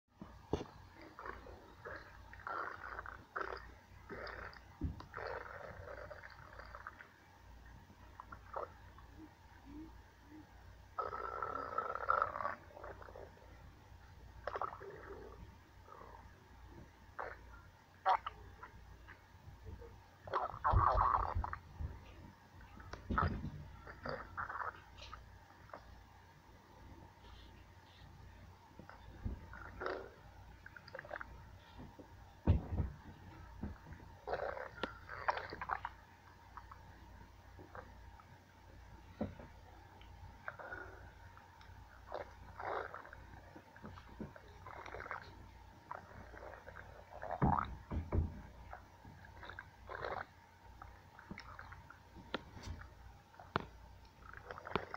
Luan stomach growls
Love it when she touches her stomach while it's growling.